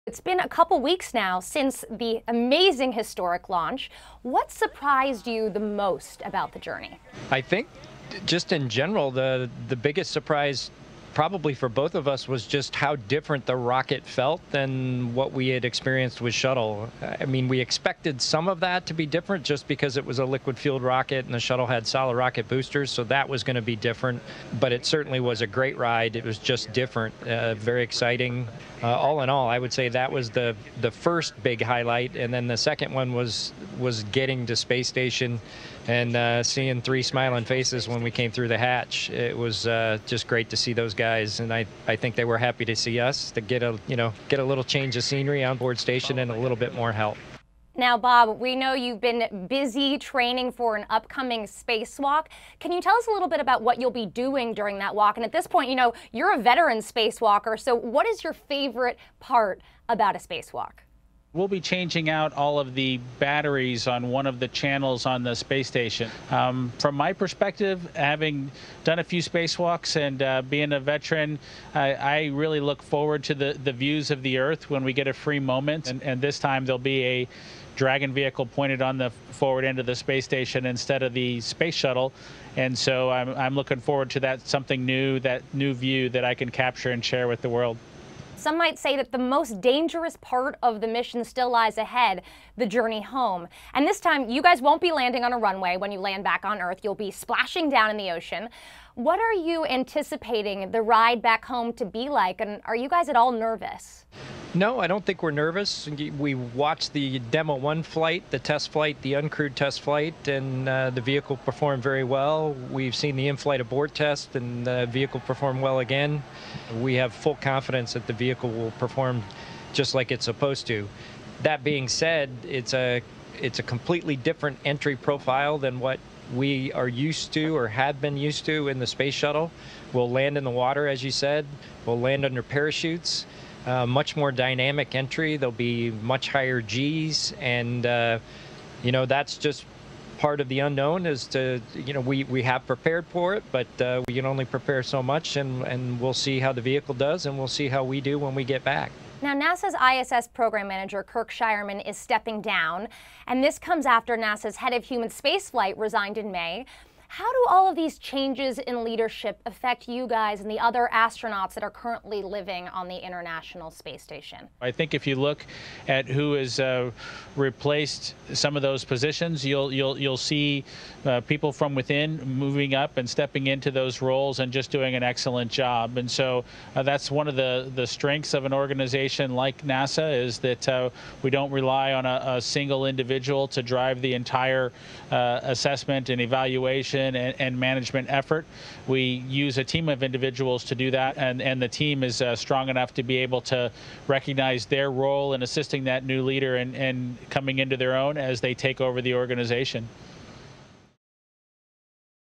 SpaceX Crew Dragon astronauts from space: It was an 'exciting' ride
Veteran NASA astronauts Bob Behnken and Doug Hurley arrived at the International Space Station aboard SpaceX's Crew Dragon. They speak to CNN Business' Rachel Crane from the ISS about the launch, spacewalks and the journey home.

Producer: Bronte Lord
Supervising Producer: Logan Whiteside

CNN Business brings you the latest news about the companies, personalities, and innovations that are driving business forward.